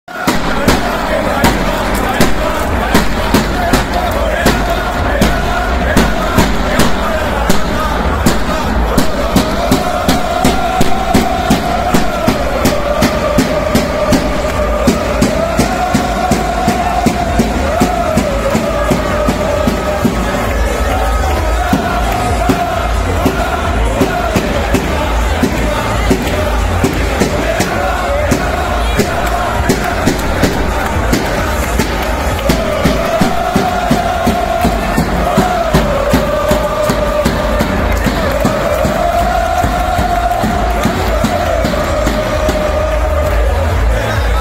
Aficionados del Valencia en la fan zone instalada en la Avenida de la Raza
Ambiente festivo en la zona reservada para la afición del Valencia en a previa de la final de la Copa del Rey en el Benito Villamarín.